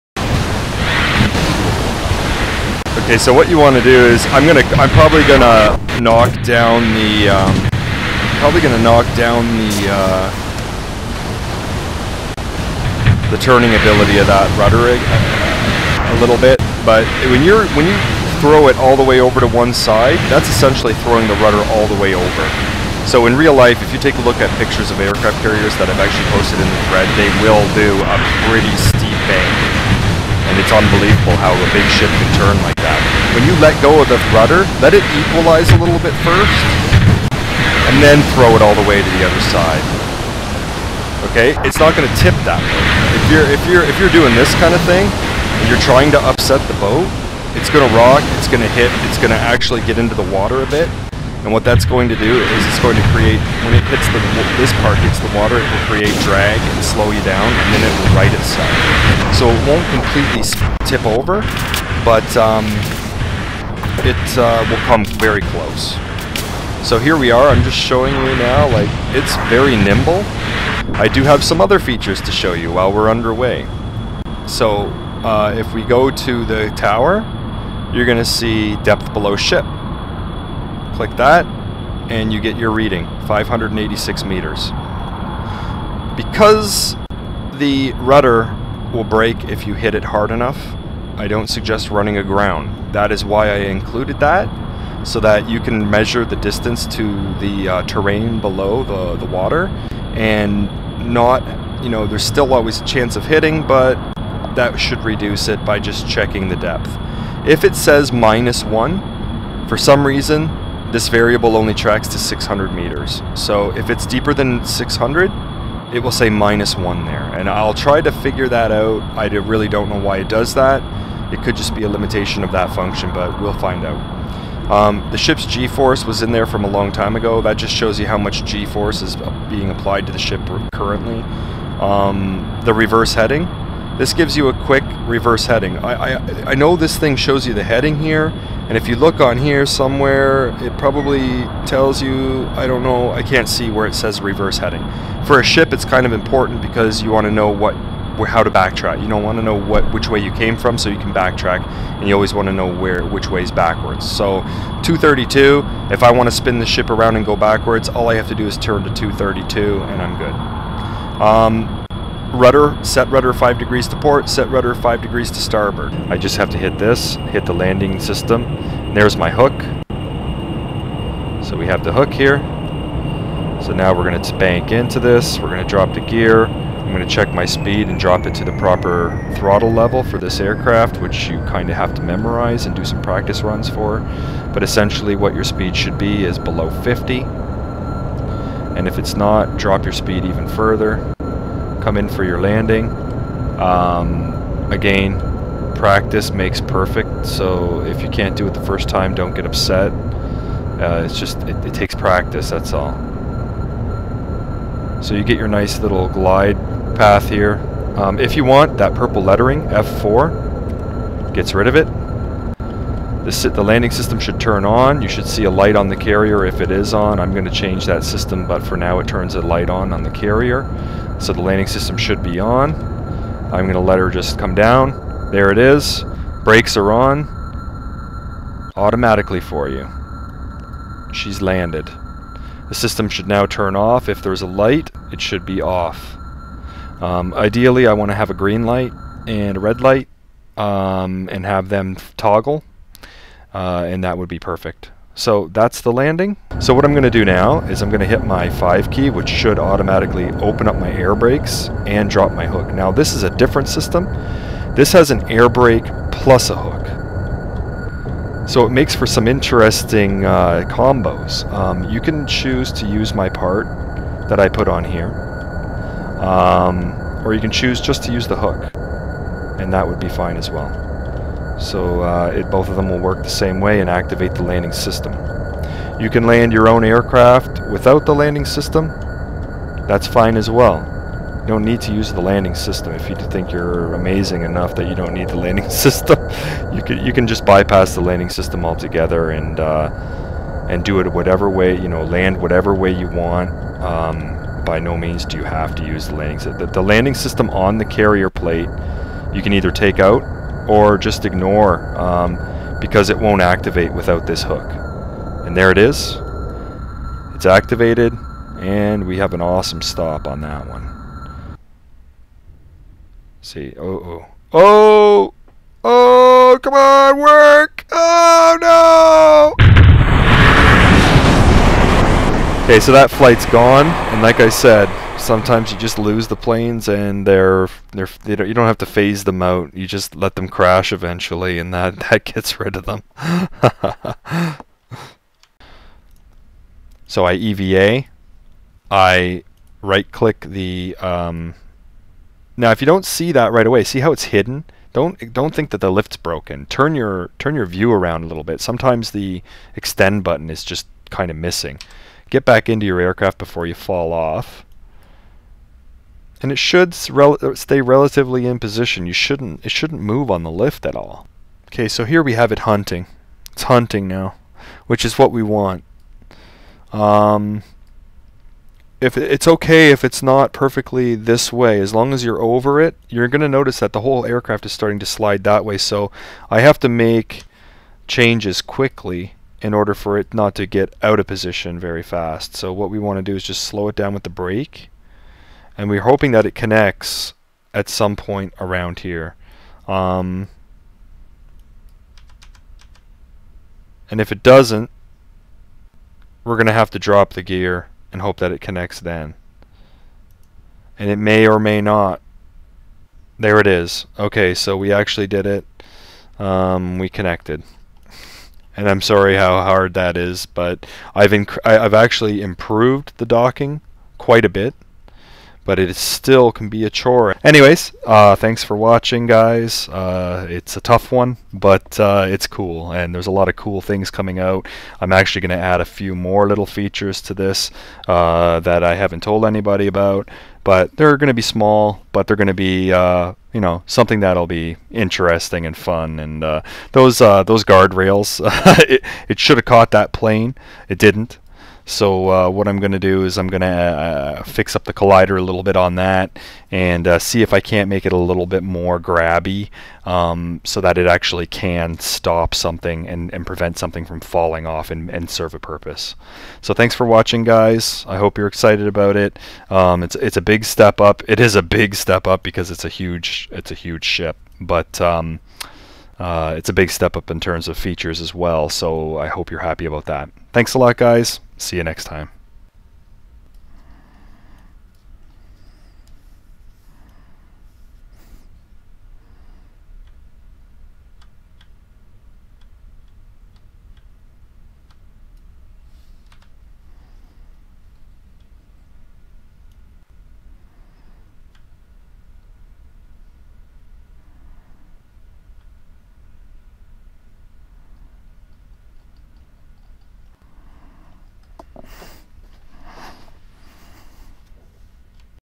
Carrier Parts R3 KCVN Pt.2 - Nuclear Carrier Preview
In continuation from Pt.1 - Covers more upcoming features, and some game play footage.

As mentioned with Pt.1, all the textures are temporary for prototyping.